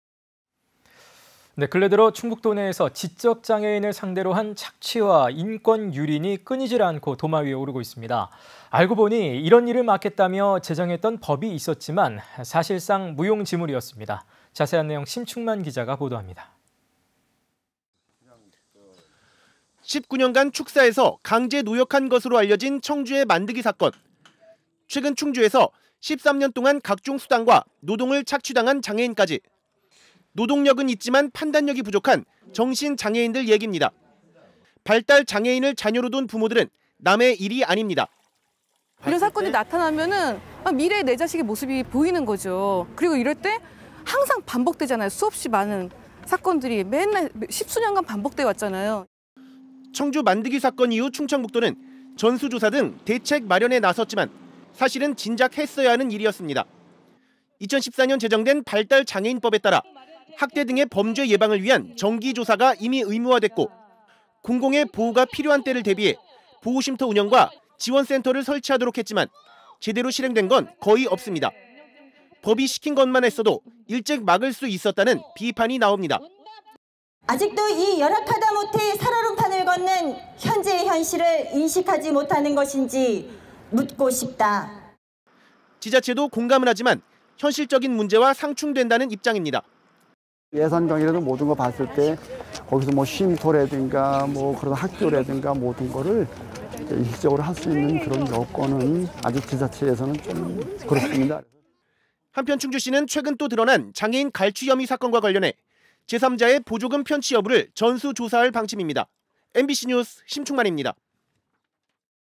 MBC충북 NEWS 161024 장애인 착취, 있는 법도 유명무실
◀ＡＮＣ▶
 충북 도내에서 지적 장애인을 상대로 한
착취와 인권유린이 연일 도마에 오르는데요,
알고보니 이런 일을 막겠다며 제정한
법이 있었는데 사실상 무용지물이었습니다.
           ◀ＥＮＤ▶

 19년간 축사에서 강제 노역한 것으로 알려진
청주의 만득이 사건.

 최근 충주에서 13년 동안
각종 수당과 노동을 착취당한 장애인까지.

 노동력은 있지만 판단력이 부족한
정신 장애인들 얘기입니다.

 발달 장애인을 자녀로 둔 부모들은
남의 일이 아닙니다.

           ◀ＩＮＴ▶
 "훗날 내 아이가 닥칠 일이구나"

 청주 만득이 사건 이후 충청북도는
전수조사 등 대책 마련에 나섰지만,
사실은 진작 했어야 하는 일이었습니다.

 2014년 제정된 발달장애인법에 따라
학대 등의 범죄 예방을 위한
정기 조사가 이미 의무화됐고,,,

 공공의 보호가 필요한 때를 대비해
보호쉼터 운영과 지원센터를 설치하도록 했지만
제대로 실행된 것은 거의 없습니다.

 법이 시킨 것만 했어도
일찍 막을 수 있었다는 비판이 나옵니다.

           ◀ＳＹＮ▶
  "예산부족 등을 이유로 미루고 있다"

 지자체도 공감은 하지만
현실적인 문제와 상충된다는 입장입니다.

           ◀ＩＮＴ▶
 "차근차근 예산 수립해서"

 한편 충주시는 최근 또 드러난
장애인 갈취 혐의 사건과 관련해,
제3자의 보조금 편취 여부를
전수 조사할 방침입니다.
 MBC뉴스 심충만입니다.//(영상취재 경석원)